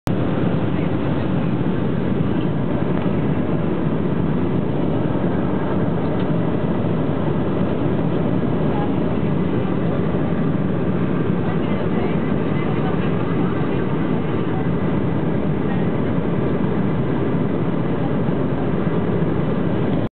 Vuelo crucero Panama - El Salvador TACA TA326
Volando a 35000 pies en  vuelo crucero ruta el Panama (PTY) - El Salvador (SAL) Avión Embraer E190 vuelo Taca TA326 operado por Avainca